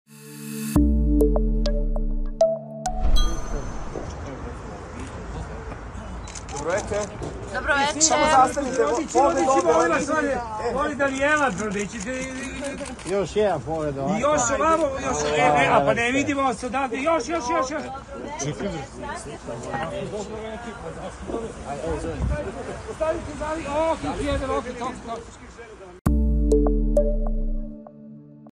RODIĆI STIGLI KOD CECE NA SLAVU Sveti Nikola u domu Ražnatovića: Bogdanin otac doneo veliki poklon
Pevačica Svetlana Ceca Ražnatović i njena porodica danas slave Svetog Nikolu, a gosti su u večernjim časovima počeli da stižu u njihov porodični dom u Ljutice Bogdana na Dedinju.